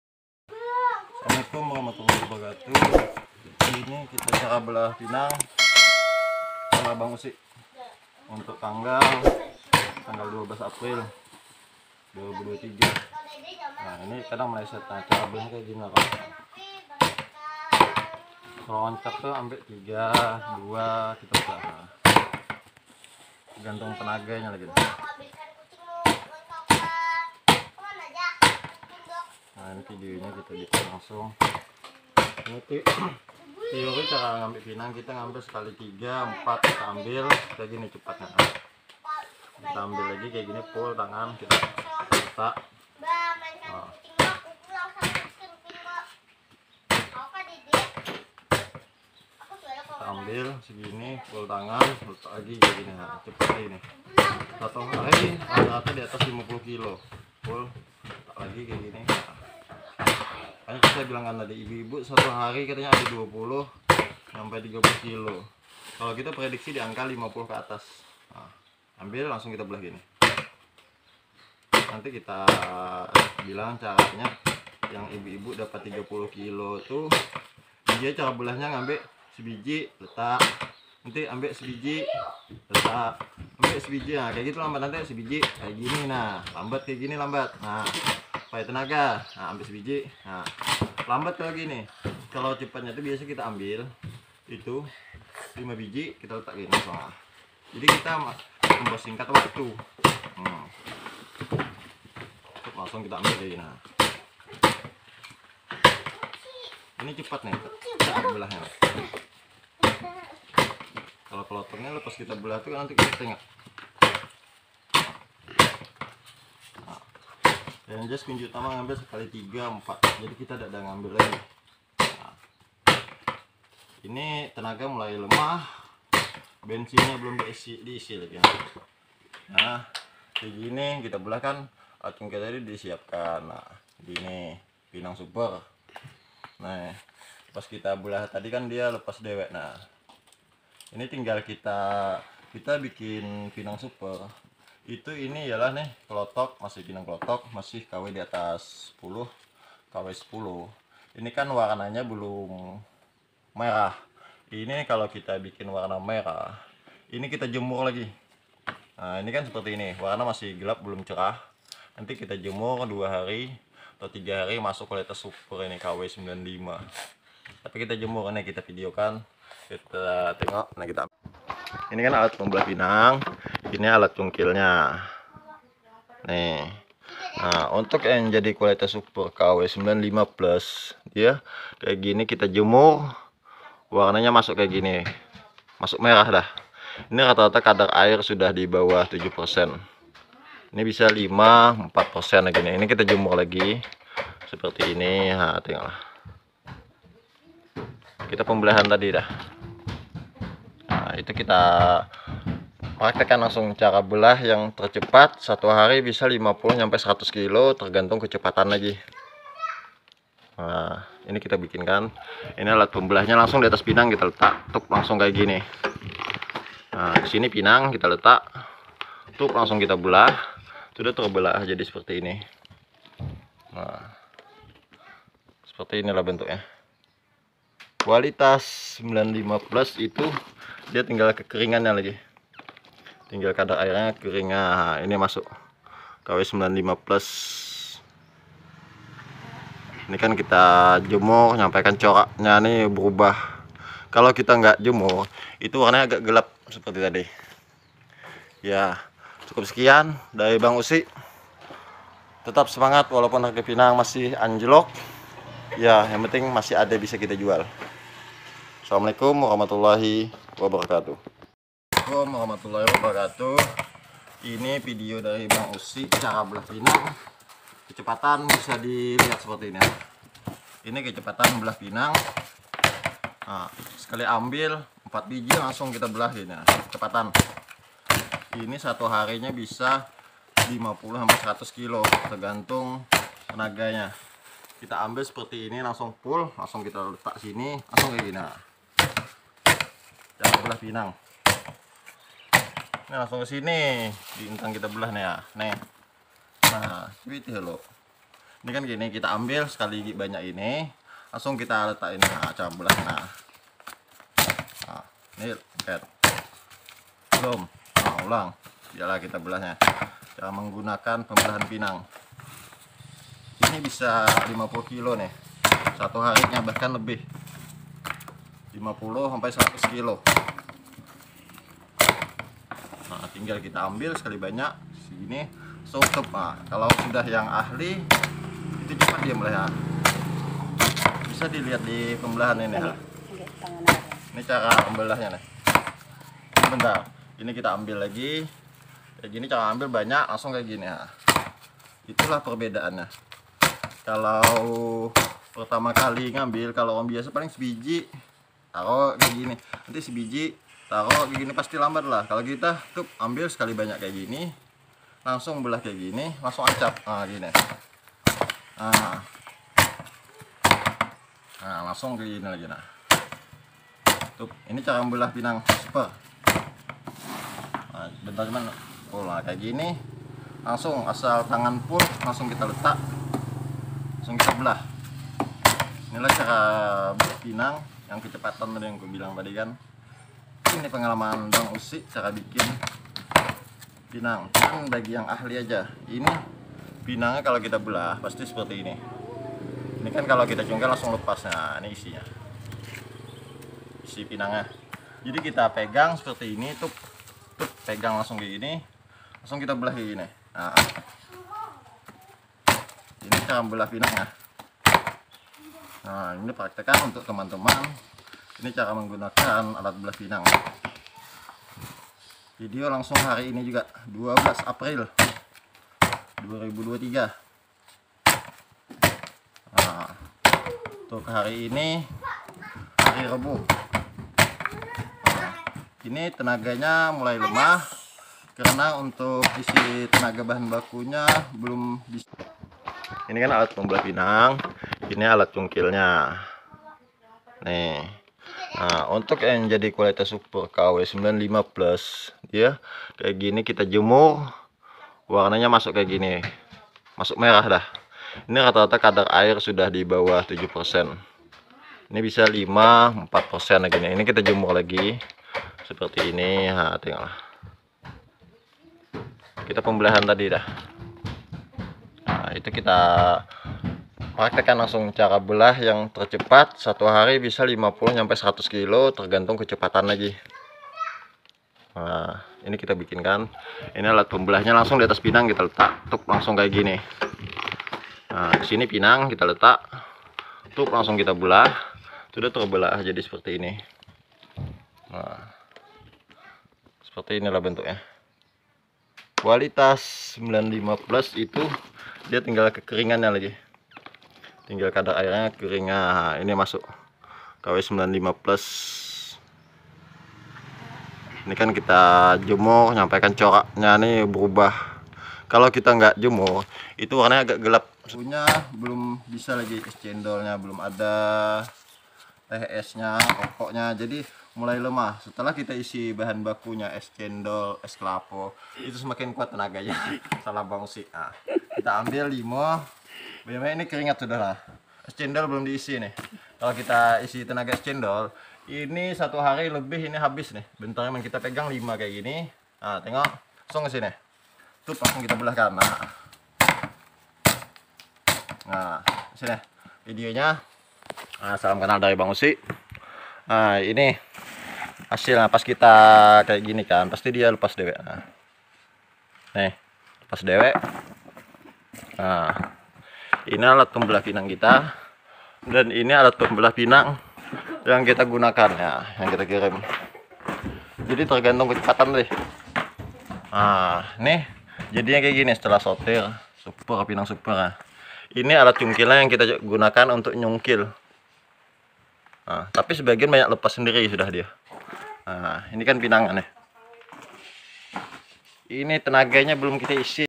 Cara belah pinang manual dengan cepat@NovanAChannel
Membelah pinang dapat dilakukan dengan menggunakan alat belah pinang manual. Alat belah pinang manual ini dapat dibuat sendiri atau membeli alat belah pinang yang sudah siap pakai.